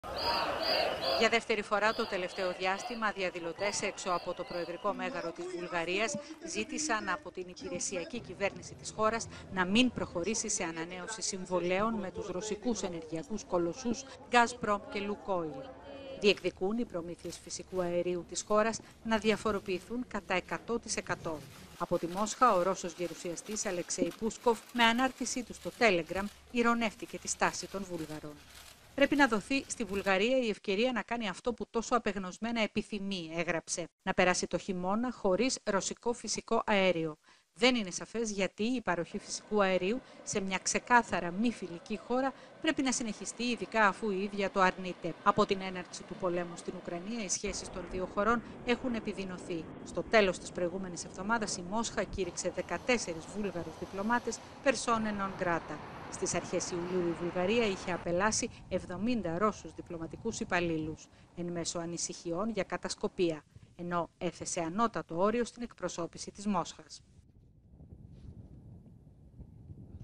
Βουλγαρία: Διαδήλωση κατά της Gazprom - Ειρωνείες από τη Μόσχα: «Ας τους δώσουμε αυτό που επιθυμούν»
Διαδηλωτές στη Σόφια διεκδικούν 100% αλλαγή στις προμήθειες φυσικού αερίου.